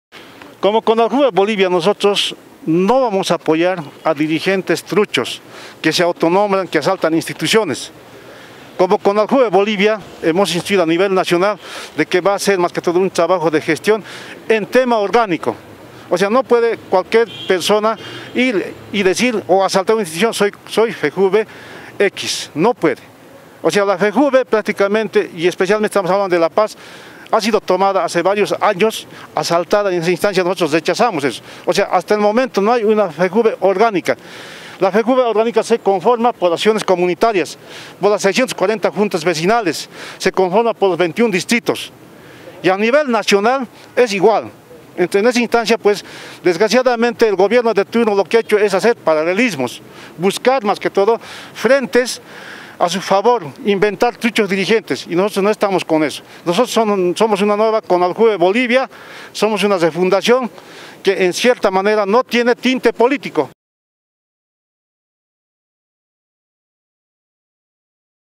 CONALJUVE ASEGURA NO RECONOCERÁ NINGUNA FEJUVE PACEÑA QUE TENGA INTROMISIÓN POLÍTICA
La Confederación de Juntas Vecinales de Bolivia CONALJUVE asegura que no reconocerá ninguna FEJUVE paceña que tenga intromisión política